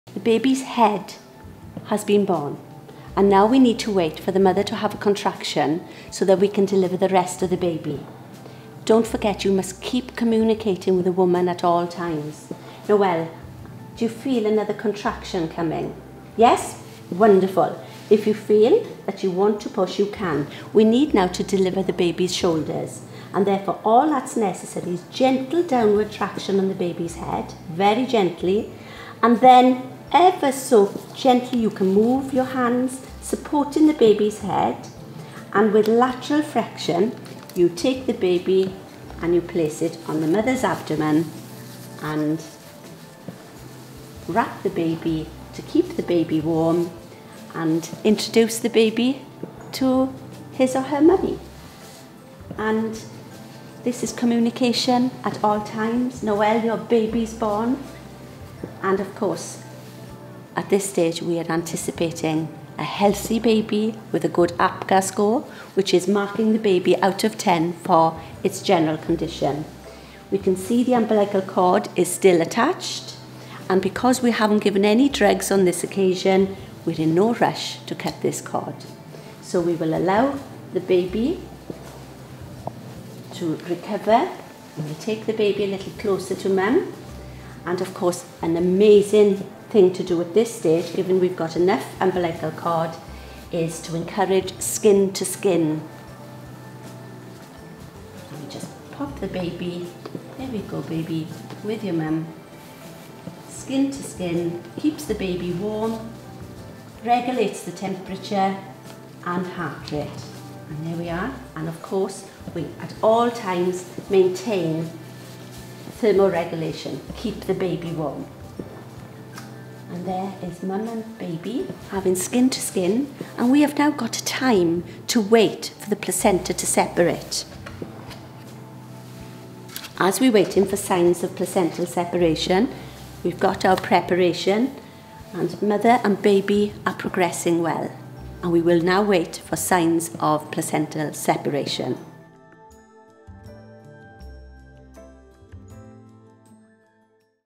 How to deliver a baby, part two - study midwifery
Students on the midwifery degree learn how to deliver a baby using high fidelity simulators.

Presented by Janine Wyn-Davies, Professional Head of Midwifery Education at the University of South Wales.